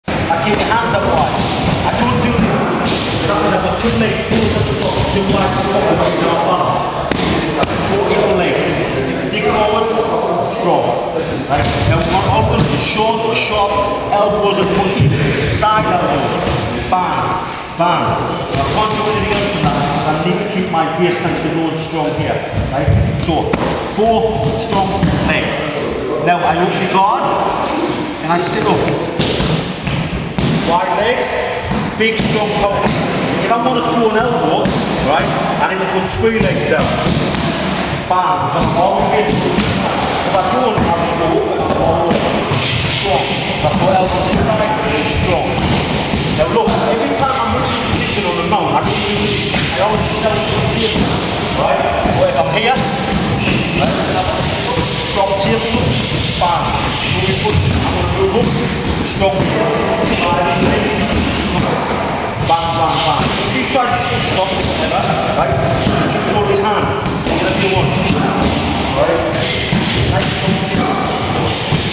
Shinsei MIxed Martial Arts Club
This is Ian 'The Machine' Freeman at our club in Liverpool 28th Feb 10